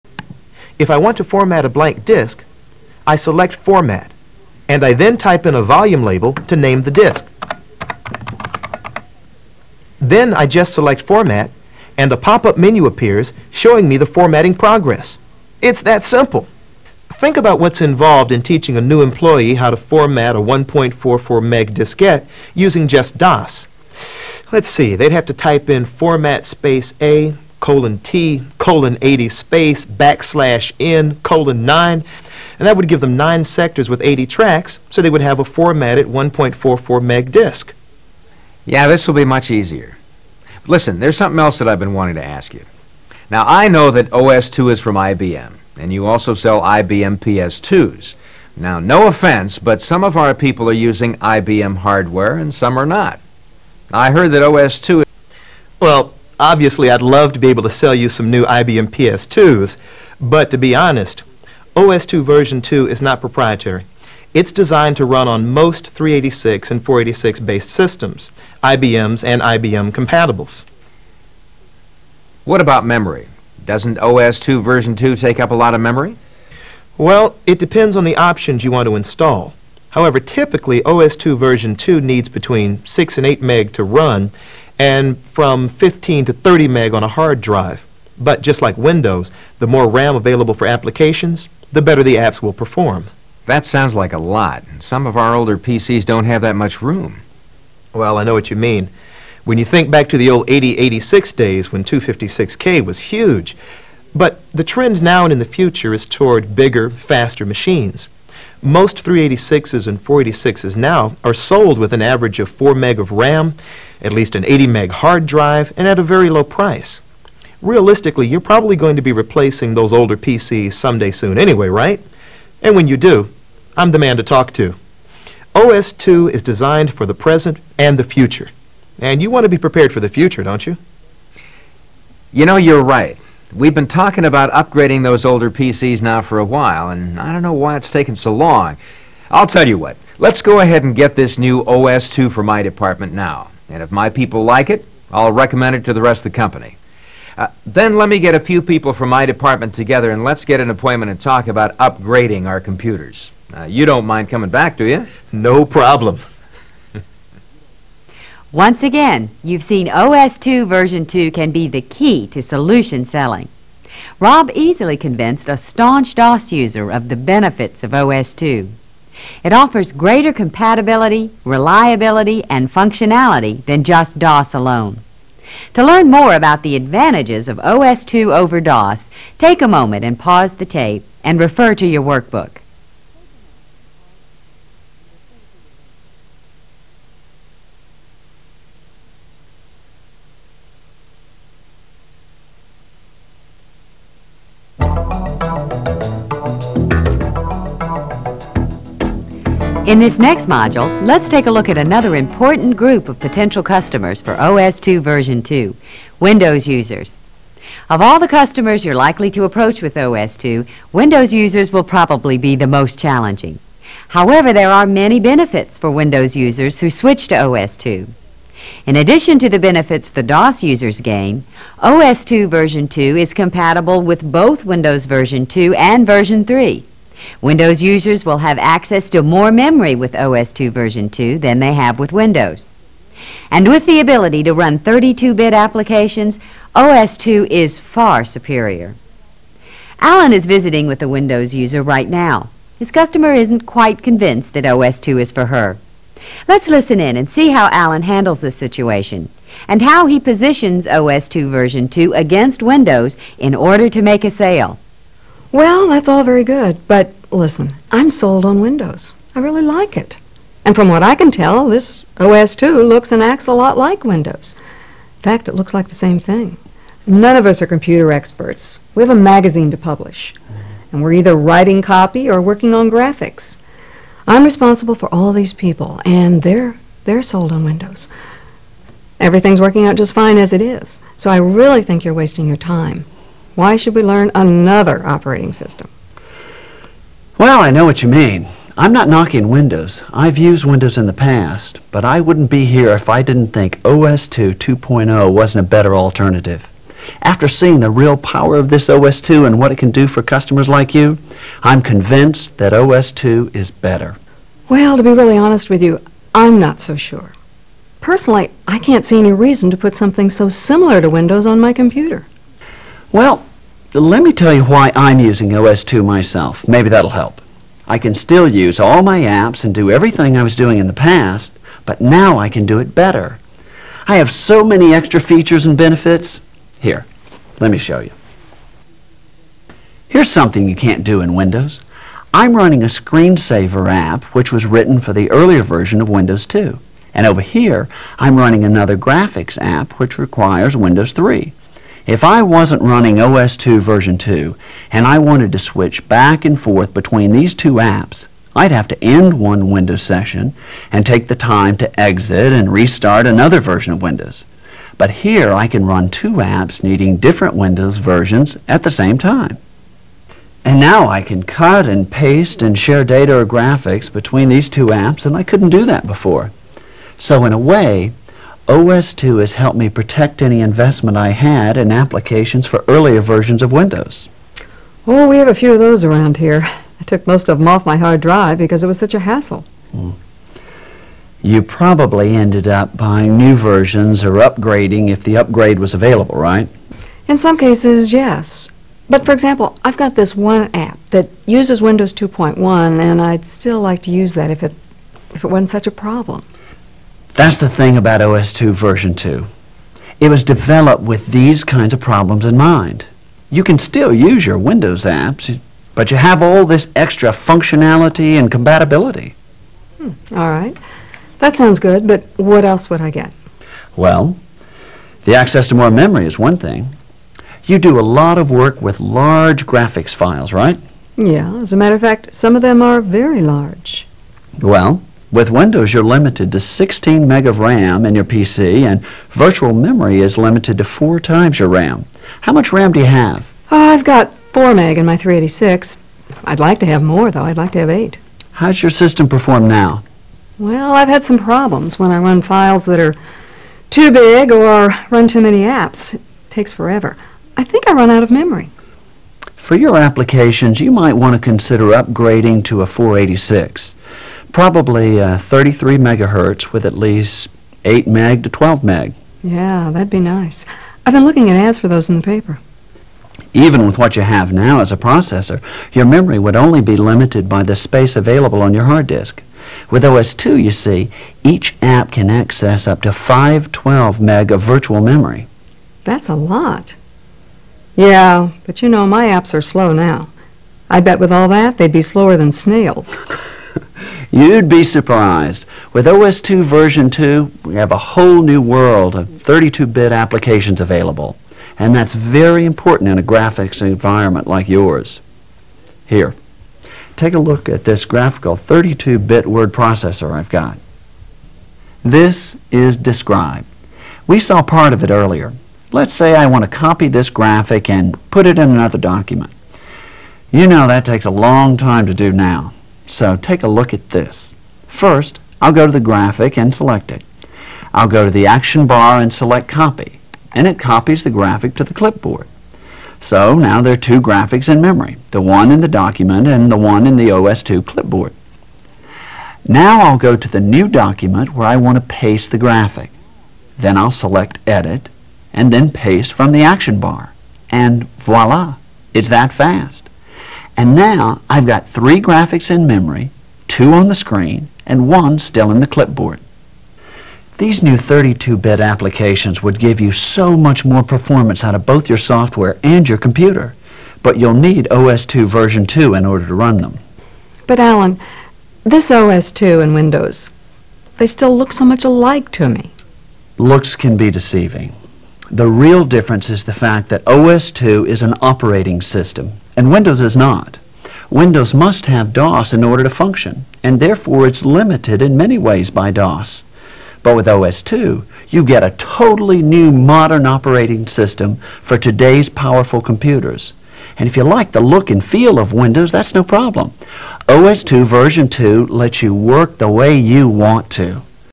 IBM Selling OS/2 Version 2 (Part 5 of 6)
15 year old training video made by IBM for marketing OS/2 Version 2 to customers. A nostalgic step back in time for the would-be competitor to Microsoft Windows.